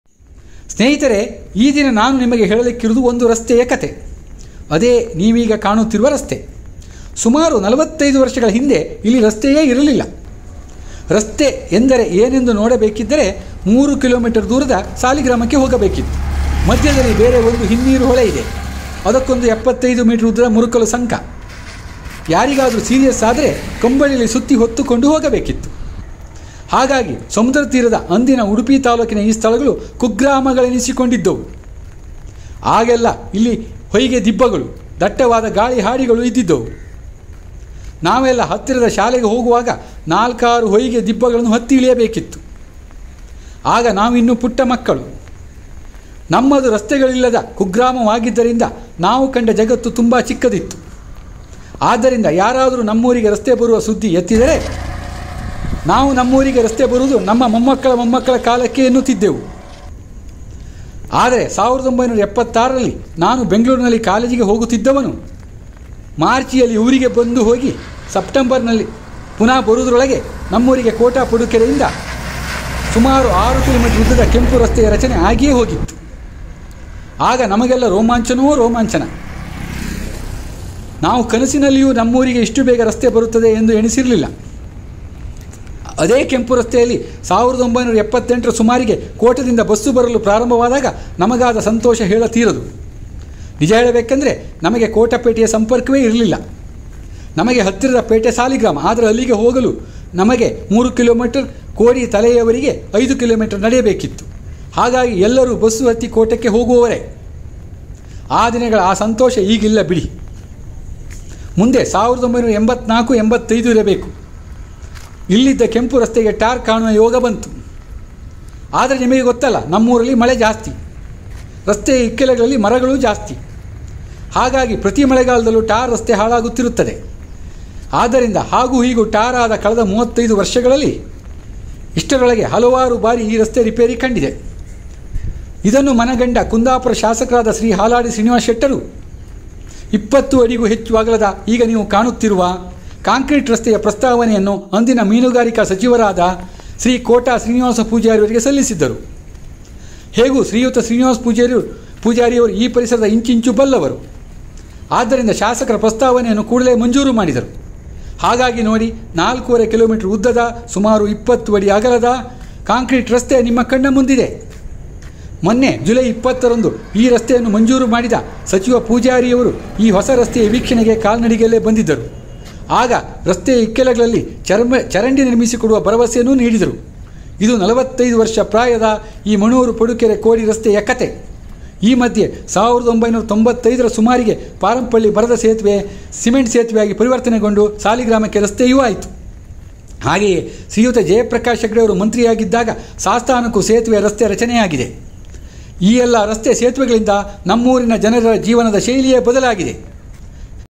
Story of a Road aged 45 / ನಲವತ್ತೈದು ವರ್ಷ ಪ್ರಾಯದ ಒಂದು ರಸ್ತೆಯ ಸತ್ಯ ಕತೆ / ಇದು ಅಂದಿನ ನಮ್ಮ ಕತೆಯೂ ಹೌದು /
Yes, this is the real story of us, not only a story of a road constructed forty five years' ago, Then our village had no roads at all, We had to go to Saligrama, a place of Gurunarasimha Temple, which is 3 kilo metres far from our place, to see what is a road, No cement bridge ( only wooden Bridge ) at Parampalli, Also at Sastan, We were small Children then, we used to tell road could be made available in our Grand Children period, as there were Sand Hills and Casuarinas tree forest in our and near by villages, But, for our surprise, road constructed in 1976 from Manur Padukere to our village Kodi Kanyana, then Udupi taluk and Dakshina Kannada district, now Brahmavara taluk and Udupi district, Made Tar road in 1985, Ours is a heavy rainy place, So, to be repaired every year, So, Kundapura MLA Sri Halady Srinivasa Shetty recommended or sent Proposal for Concrete road of 4.5 km long and 20 feet width, to then Fisheries Minister Sri Kota Srinivasa Poojary, He sanctioned immediately, Now road is ready and Cabinet Minister visited the road on 20th July 2021, In 1995, construction of Cement Bridge was also made substituting Wooden Bridge in Parampalli along with a connecting road to Saligrama, When Sri Jayaprakash Hegde, present President of Backward Class Commission, was district minister of undivided Dakshina Kannada, a bridge at Kodi was built with connecting road to Sastan, This is also the real story of then situation of our village and not only a story of a road,
Watch Video of Chandrashekara Navada

ಅಂದು ನಮ್ಮ ಊರು ಕುಗ್ರಾಮವಾಗಿತ್ತು, ಇಂದು ಉತ್ತಮ ರಸ್ತೆ, ಹಲವಾರು ಸಿಮೆಂಟ್ ಸೇತುವೆಗಳ ರಚನೆಯಾಗಿ ಅಭಿವೃದ್ಧಿಗೊಂಡಿದೆ. ಅದರ ಕತೆಯೇ ಈ ವೀಡಿಯೋದಲ್ಲಿದೆ.